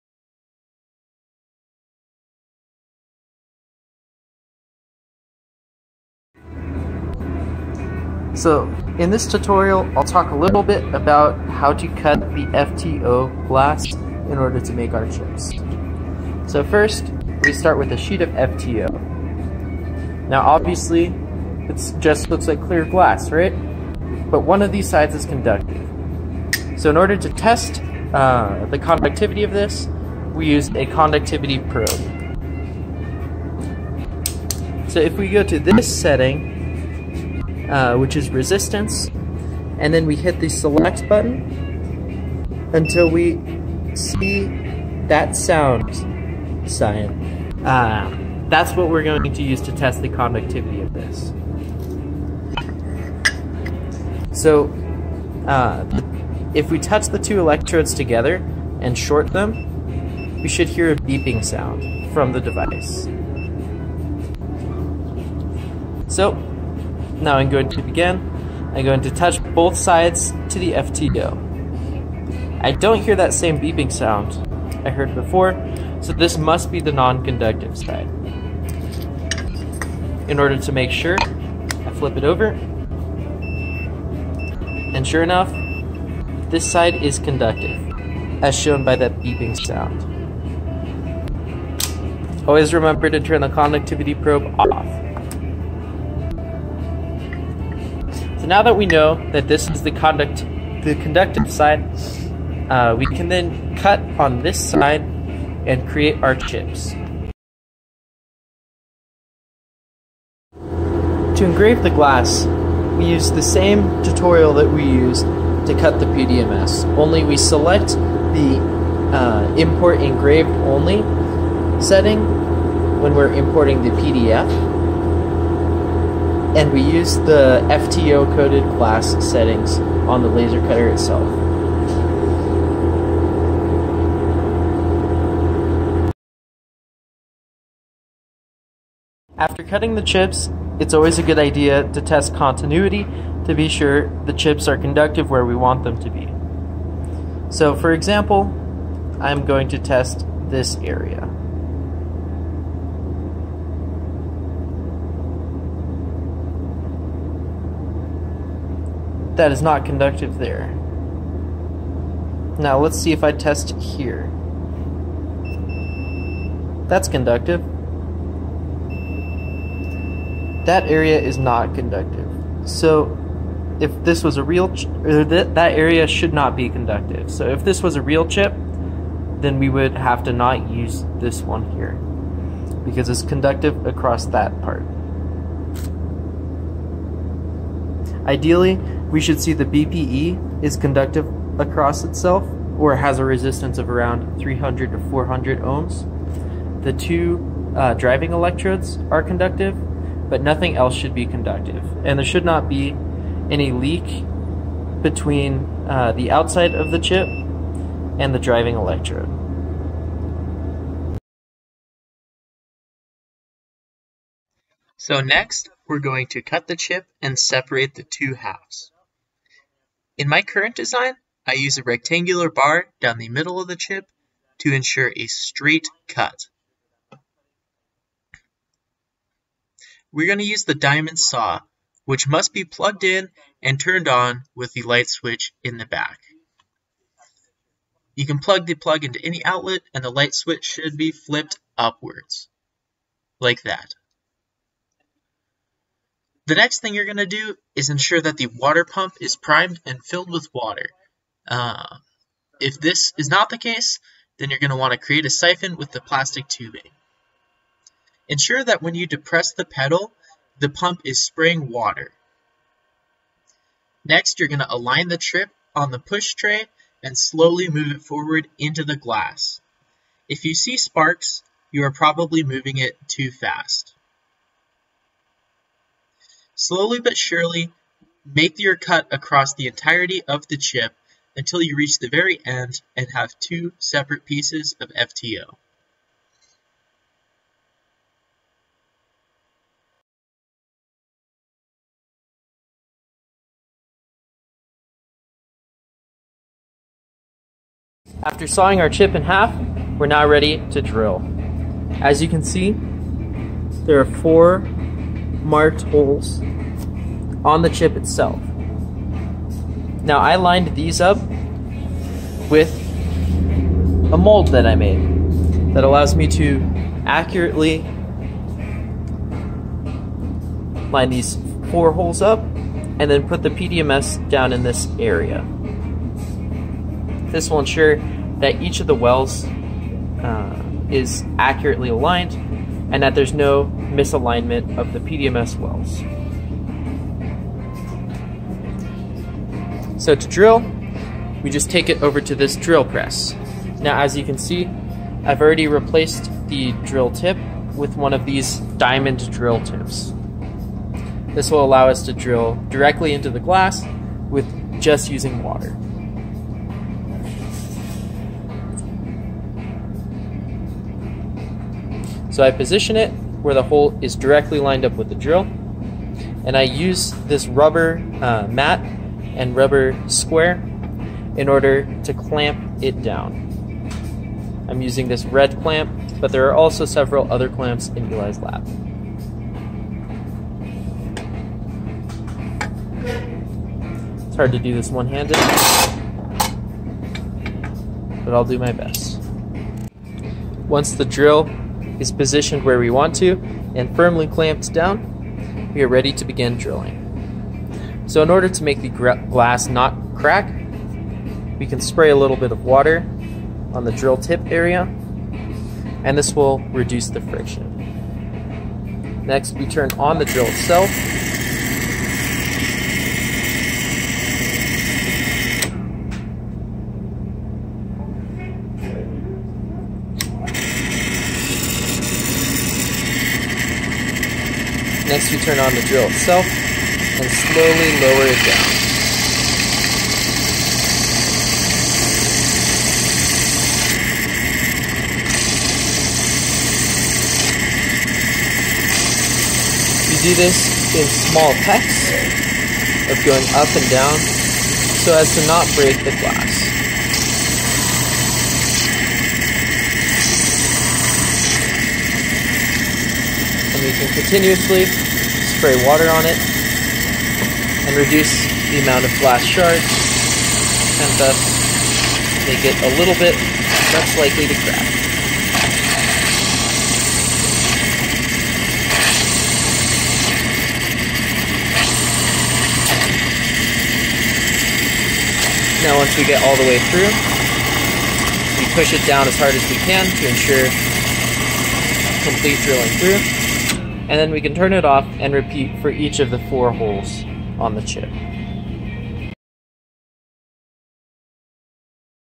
SOP - BPE ECL Chip Fabrication (Gen 1) - Modifying FTO Glass
Nick describes how to identify which side of FTO glass is conductive and how to laser pattern the surface to create bipolar electrode chip arrays.